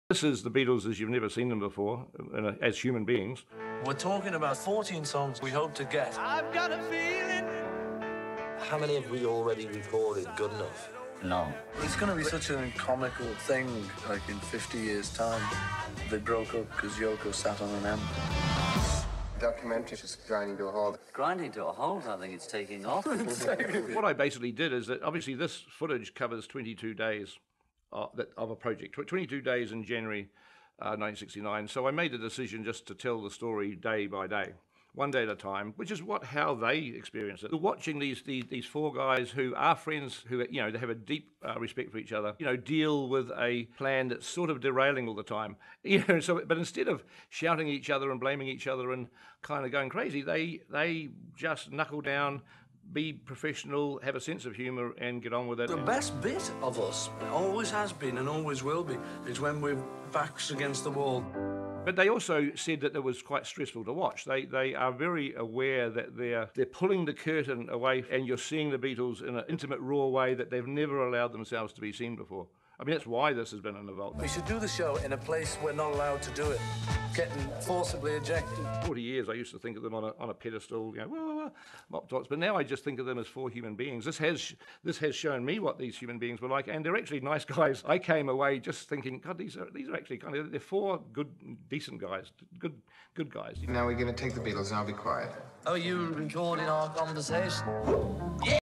ビートルズの新ドキュメンタリー配信へ、人間としての4人描く秘蔵映像
「これは、今まで誰も見たことのない人間としてのビートルズだ」――彼らの新たなドキュメンタリー「ザ・ビートルズ：Get Back」が、ディズニープラスで配信される。50年間封印・保管されていた57時間分の秘蔵映像を編集したもので、監督はピーター・ジャクソン。